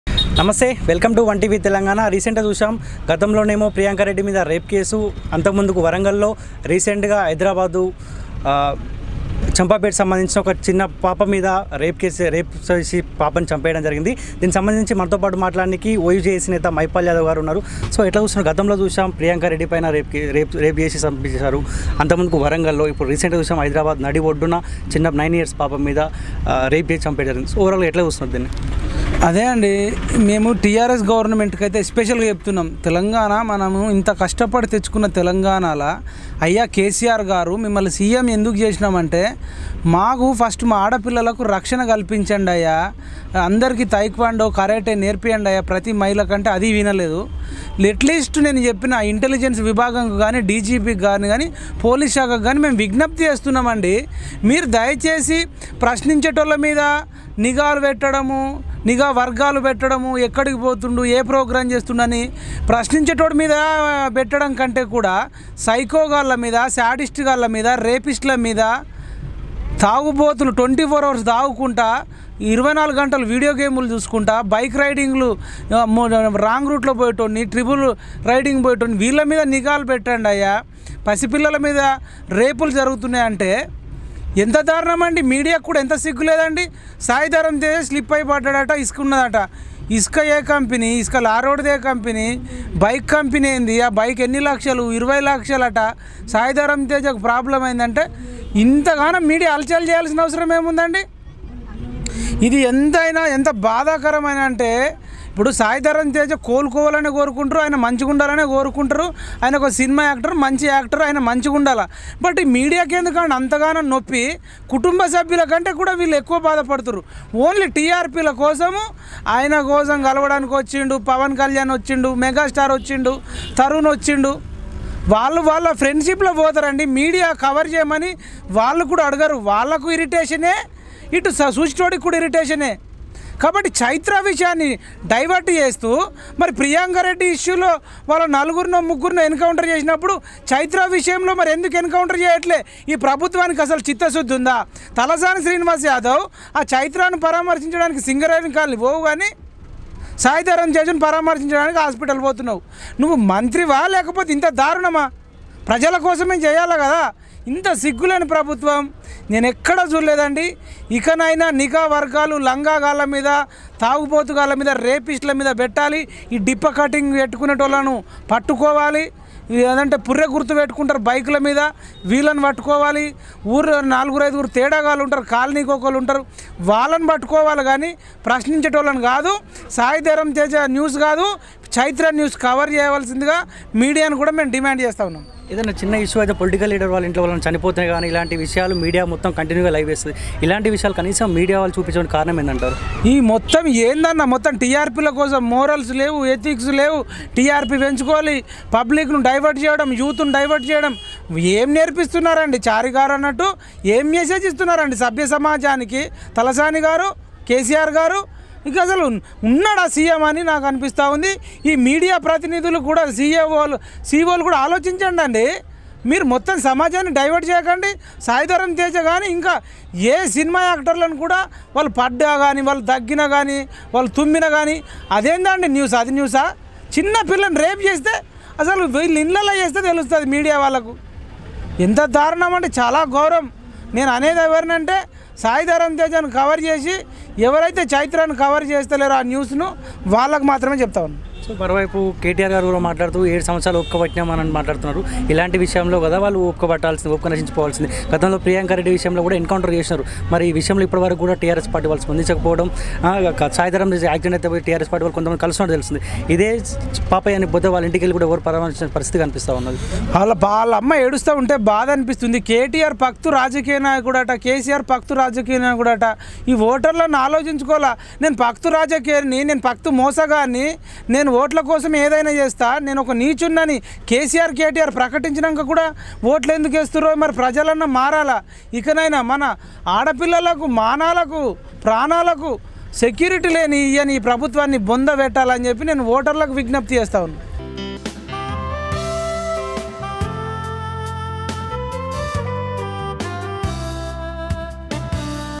OU Leader Mahipal Yadav about 6 Years Baby Incident | Singareni Colony | Chaitra | One TV Telangana
OU Leader Mahipal Yadav about 6 Years Baby Incident
 Singareni Colony
Chaitra
One TV